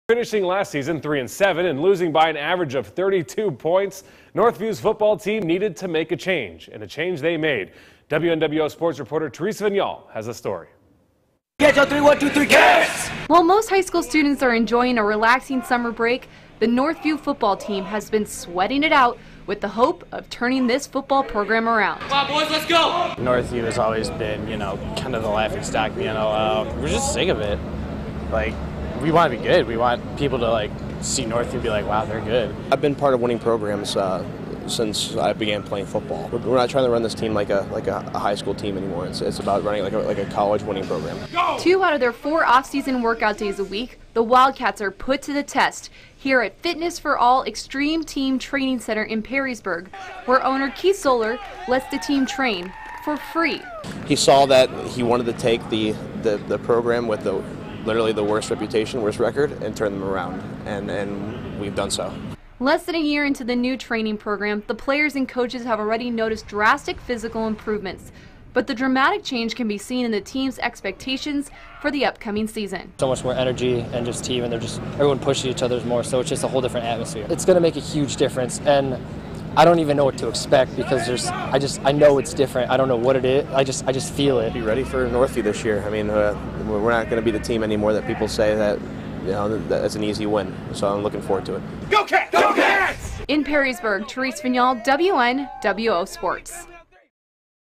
Northview looks to be the dark horse this football season
Northview high's football team hard at work this offseason